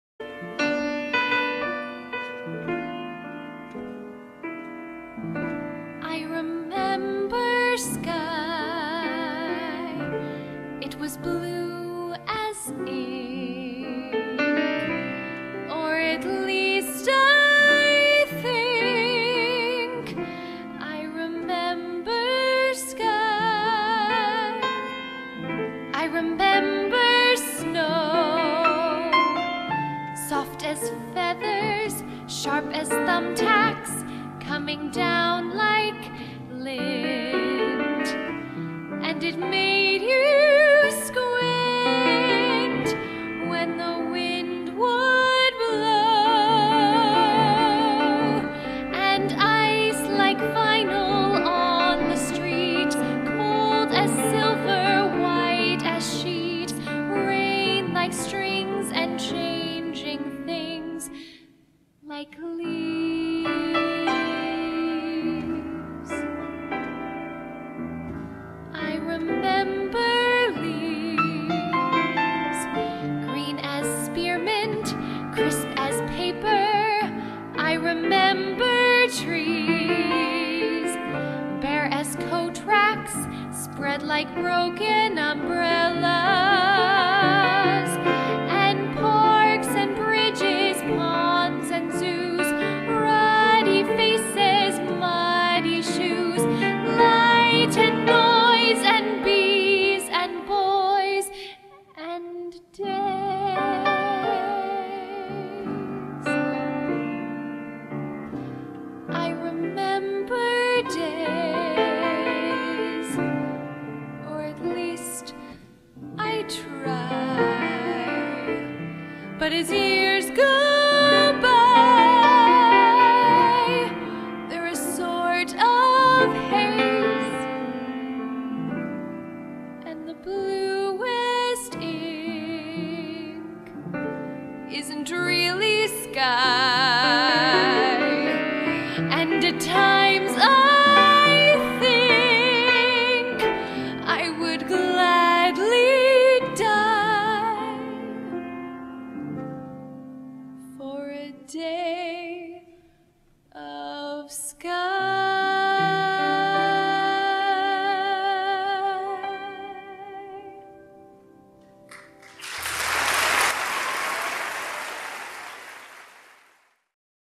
The Show in Pictures-Anne Frank
A memory the last Fall Play in the mall at Capo Valley High school. I hope you'll all enjoy this :)